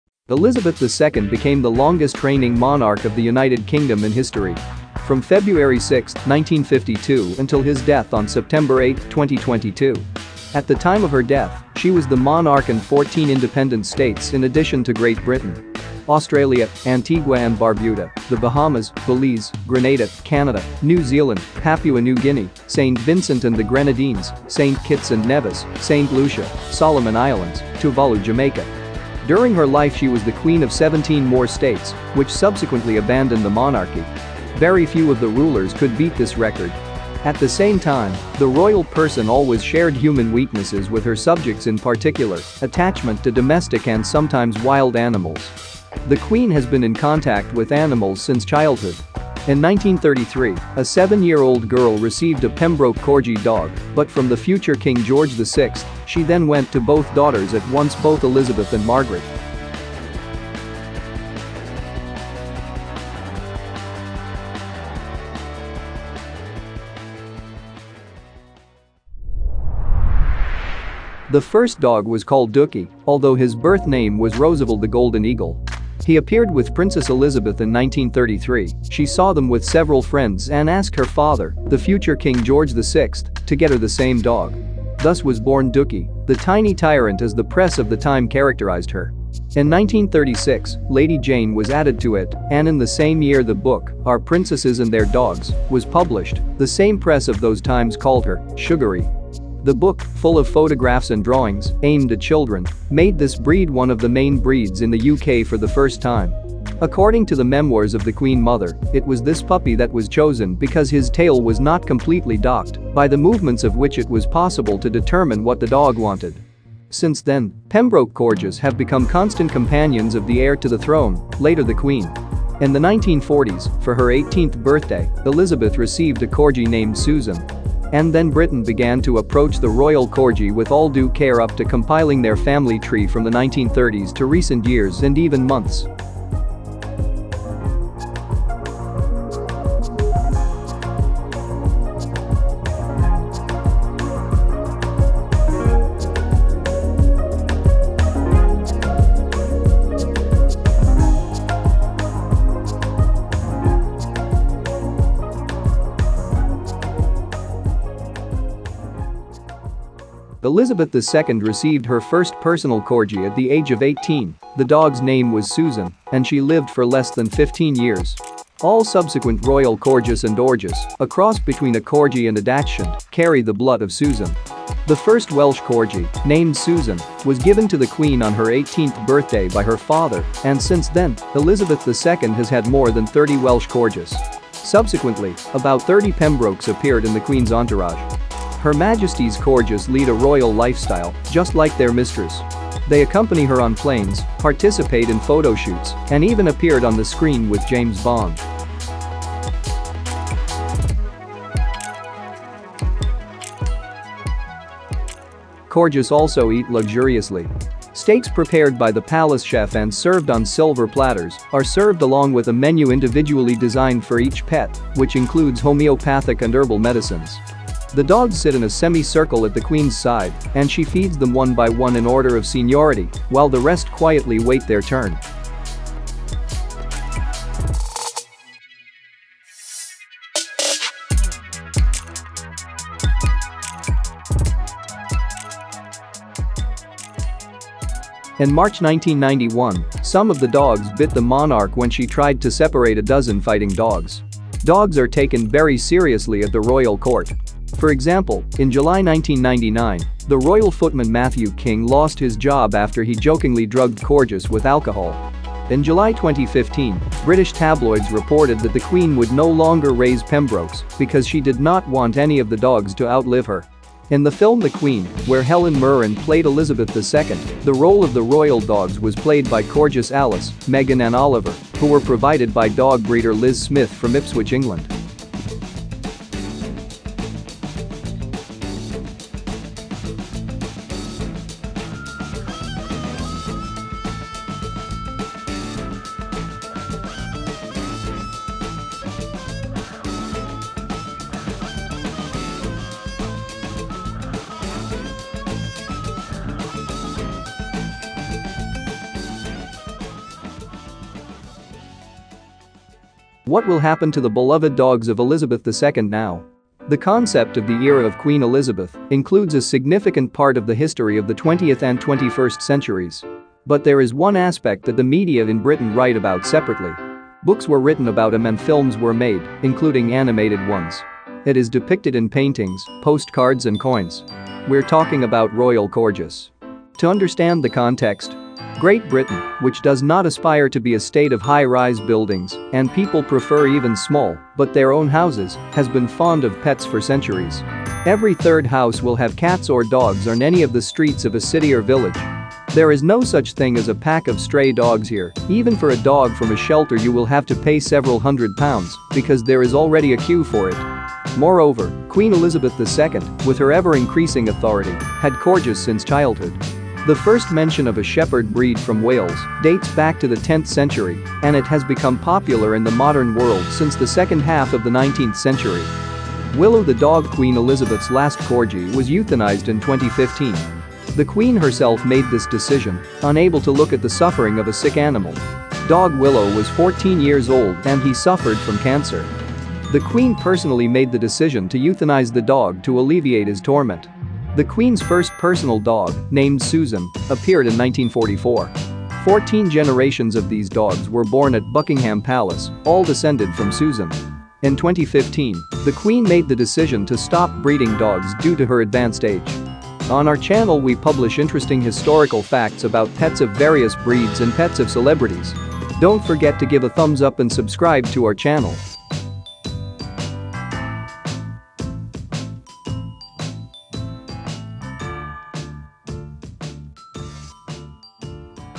Uncovering the SECRET Life of Queen Elizabeth II's Royal Corgis!
In this video, we explore the Royal Life of the Queen's Corgis - Elizabeth ll and her corgis. From her coronation to her official engagements, we'll take a look at everything the corgis have been up to!

Watch this video to get insight into the lives of the Queen's corgis, from their coronation to their official engagements. We'll explore everything from their royal family to their favorite activities!
Corgi "Dookie" Elizabeth II
The first dog was called Dookie, although his birth name was Rosavel the Golden Eagle.
He appeared with Princess Elizabeth in 1933 - she saw them with several friends and asked her father, the future King George VI, to get her the same dog. Thus was born Dookie, the "tiny tyrant" as the press of the time characterized her. In 1936, Lady Jane was added to it - and in the same year the book "Our Princesses and Their Dogs" was published (the same press of those times called her "sugary").
The book, full of photographs and drawings, aimed at children, made this breed one of the main breeds in the UK for the first time.
According to the memoirs of the Queen Mother, it was this puppy that was chosen because his tail was not completely docked, by the movements of which it was possible to determine what the dog wanted.
Since then, Pembroke Corgis have become constant companions of the heir to the throne, later the queen.
In the 1940s, for her 18th birthday, Elizabeth received a corgi named Susan. And then Britain began to approach the royal corgi with all due care - up to compiling their family tree from the 1930s to recent years and even months.
Corgi for life
Elizabeth II received her first "personal" corgi at the age of 18 - the dog's name was Susan and she lived for less than 15 years. All subsequent royal corgis and dorgis (a cross between a corgi and a dachshund) carry the blood of Susan. The first Welsh Corgi, named Susan, was given to the Queen on her 18th birthday by her father, and since then, Elizabeth II has had more than 30 Welsh Corgis.
Subsequently, about 30 Pembrokes appeared in the Queen's entourage.
Her Majesty's Corgis lead a royal lifestyle, just like their mistress: they accompany her on planes, participate in photo shoots and even appeared on the screen with James Bond. Corgis also eat luxuriously: steaks prepared by the palace chef and served on silver platters are served along with a menu individually designed for each pet, which includes homeopathic and herbal medicines.
The dogs sit in a semi-circle at the queen's side, and she feeds them one by one in order of seniority, while the rest quietly wait their turn.
In March 1991, some of the dogs bit the monarch when she tried to separate a dozen fighting dogs.
Dogs are taken very seriously at the royal court: for example, in July 1999, the royal footman Matthew King lost his job after he jokingly drugged Corgis with alcohol.
In July 2015, British tabloids reported that the Queen would no longer raise Pembrokes because she did not want any of the dogs to outlive her.
In the film The Queen, where Helen Mirren played Elizabeth II, the role of the royal dogs was played by corgis Alice, Megan and Oliver, who were provided by dog breeder Liz Smith from Ipswich, England.
Willow the dog - Queen Elizabeth's last corgi
  - was euthanized in 2015. The queen herself made this decision, unable to look at the suffering of a sick animal.
Dog Willow was 14 years old and he suffered from cancer. The Queen personally made the decision to euthanize the dog to alleviate his torment. The Queen's first personal dog, named Susan, appeared in 1944. 14 generations of these dogs were born at Buckingham Palace, all descended from Susan.
In 2015, the Queen made the decision to stop breeding dogs due to her advanced age.

Timestamps:
00:00 - 01:10 Intro The Queen and her corgis
01:10 - 02:47 Corgi "Dookie" Elizabeth II
02:47 - 03:18 Corgi "Susan" Elizabeth II
03:18 - 04:27 The Royal Life Of The Queen's Corgis
04:27 - 05:03 In the film The Queen, where Helen Mirren played Elizabeth II
05:03 - 06:05 What will happen to the beloved dogs of Elizabeth II now?
06:05 - 06:58 Willow the dog - Queen Elizabeth's last corgi